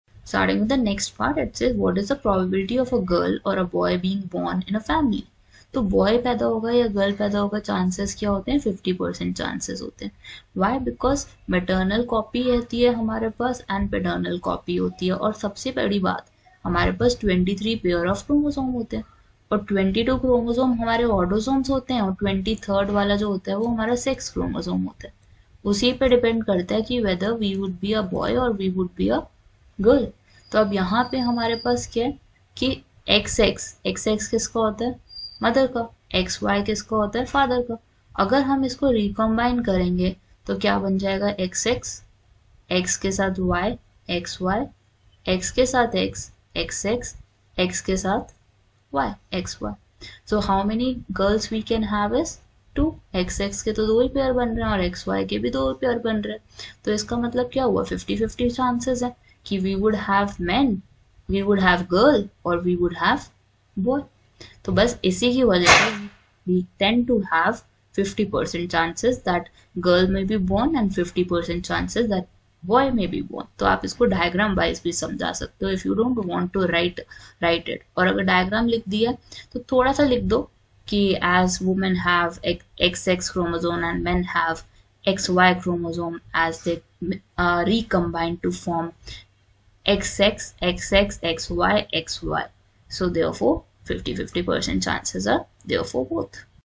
30. What is the probability of a girl or a boy being born in a family? Justify your answer
What is the probability of a girl or a boy being born in a family? Justify your answer